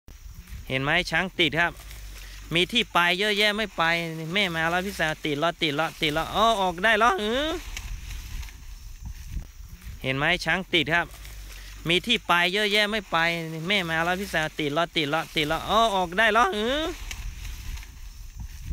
Baby Elephant's Backside Stuck Between Trees
A baby elephant misjudged the size of her backside and found herself stuck between two trees.

The two-year-old jumbo named Valentine was out for a morning walk with her handler when she tried to squeeze through the gap.

Footage shows how Valentine's rear end was wedged between the two trees in the jungle in Chiang Mai, northern Thailand on August 6.